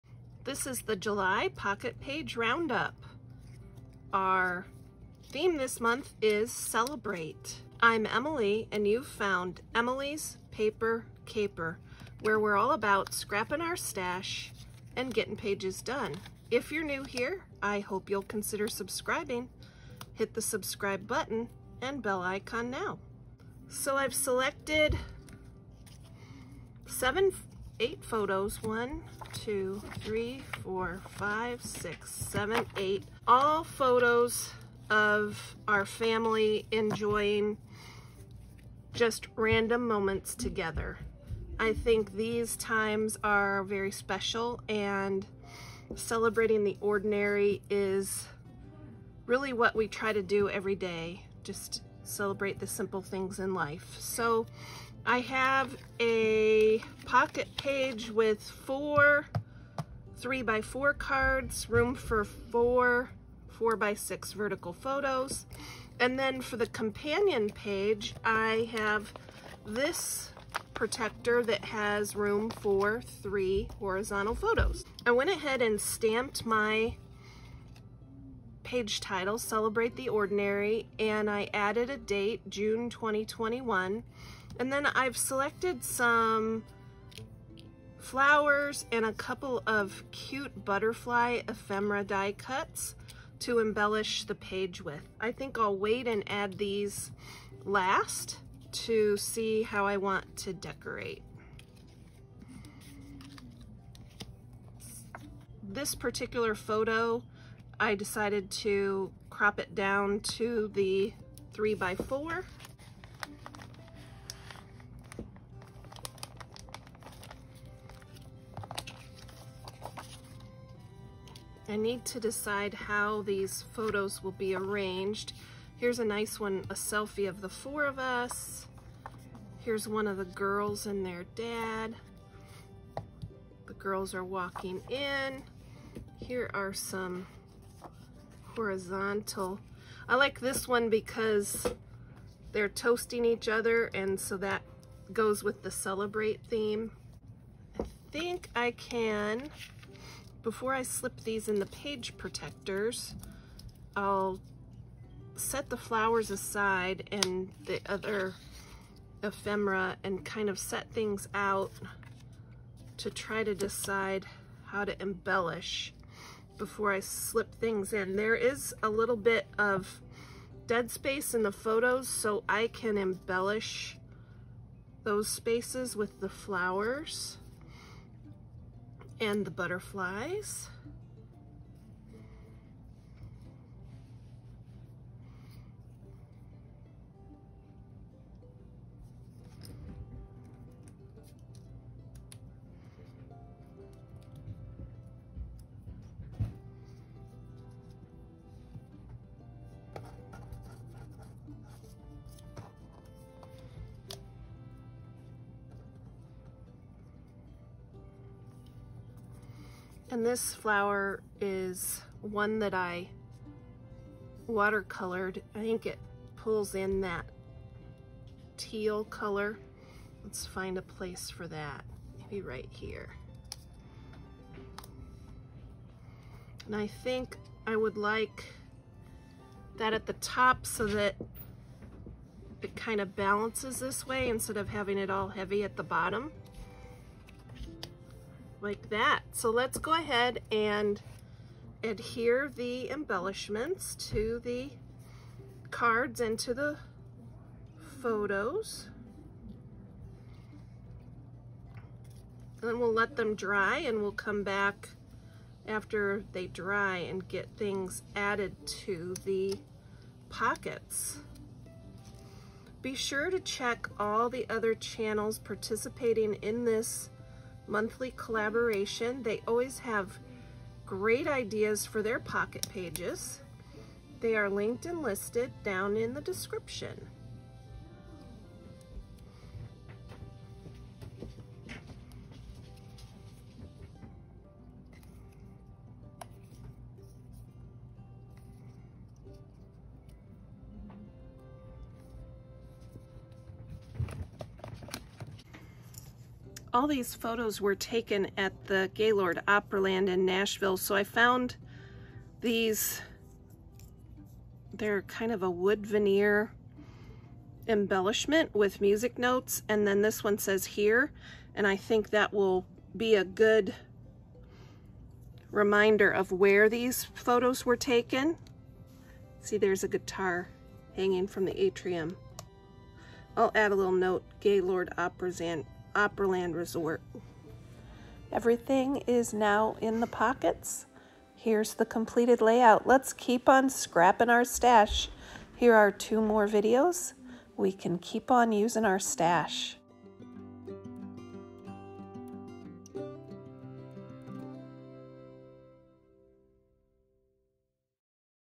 Pocket Page Scrapbook Ideas - Pocket Page Round-Up Hop - (July 2021)
This video is part of the "Pocket Page Round-Up" YouTube hop for 2021. We are a group of scrapbook channels sharing our pocket page ideas. The links to all of the participating channels are linked and listed below.

For the month of July 2021, our theme is "Celebrate." My page title is "Celebrate the Ordinary" and I'm using photos taken of our family time together on a recent trip to Nashville to visit our older daughter.